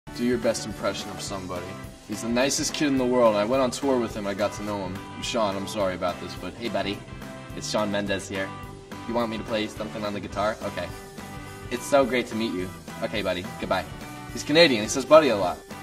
[CC] Charlie Puth doing a spot on impression of Shawn Mendes
BONUS VIDEO! It's a old video, but I wanted to make sure that you all have seen it. :P

-- English captions (CC) made by me --